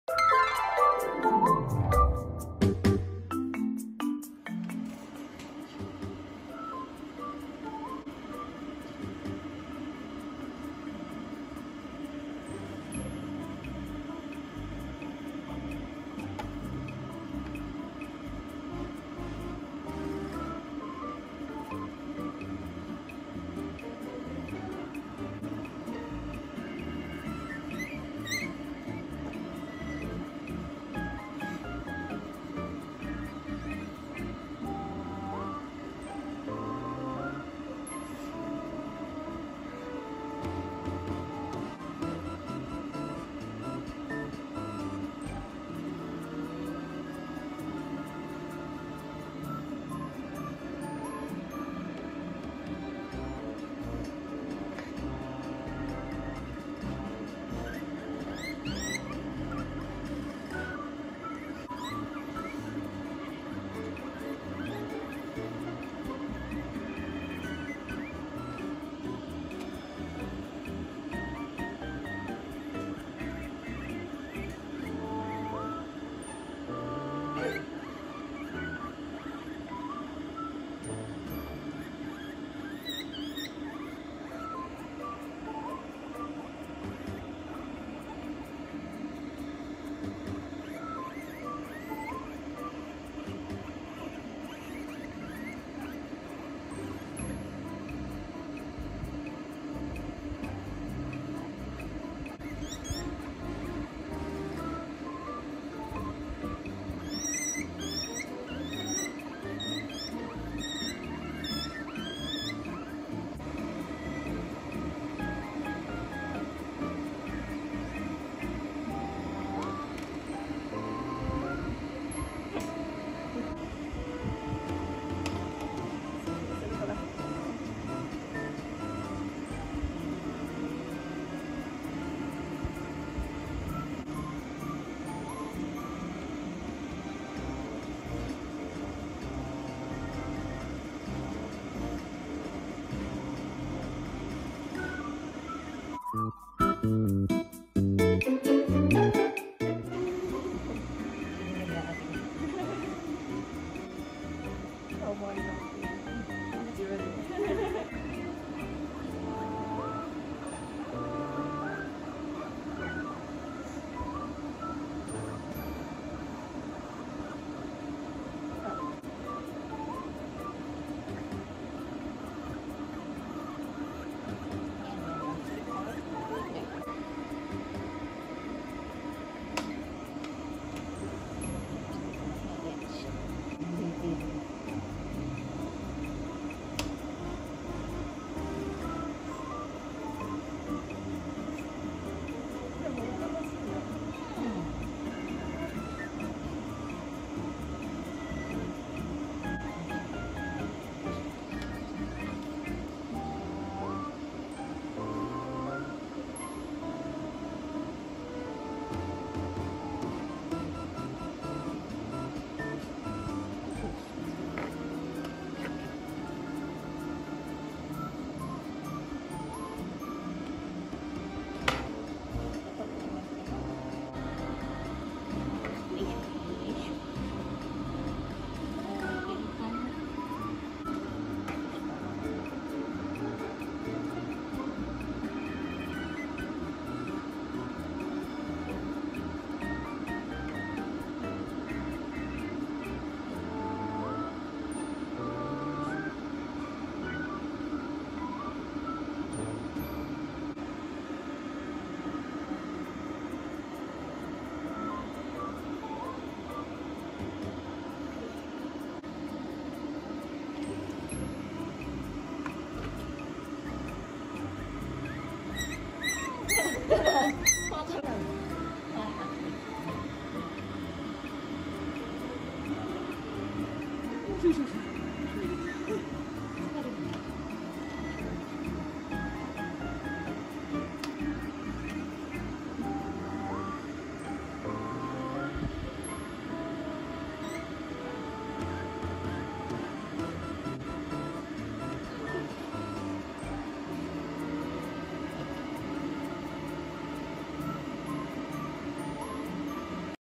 #5【モルモット】もかさん初シャンプーの乾かし編！
今回の撮影は、HANA Petさんのご好意で特別に撮影させていただいています。

イングリッシュモルモットを3匹飼っています。
ゆったり投稿していくので、よろしくお願いします。